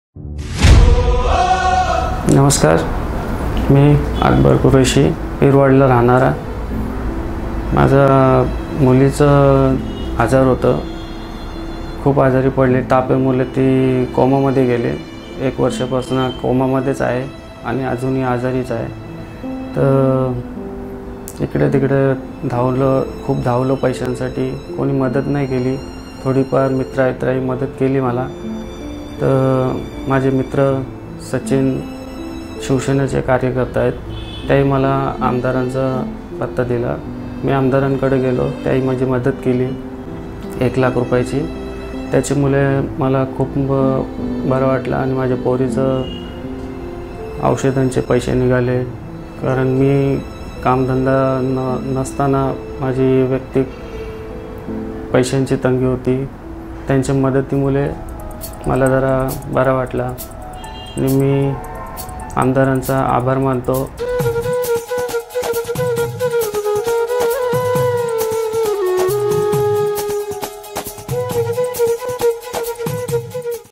एक हात मदतीचा ।भाग ०३
पिरवाडी येथील अकबर कुरेशी यांची मुलगी आजारपणामुळे कोमामध्ये गेली परिस्थिती बिकटची असल्याने मुलीच्या आजारपणावर खर्च करणे श्यक्य नव्हते अश्यावेळी आपल्या कार्यसम्राट आमदार यांनी एक मदतीचा की हात पुढे केल्याने अकबर कुरेशी यांना आपल्या मुलीच्या आजारपणात मदत होत राहिली आणि अजून नाही होते.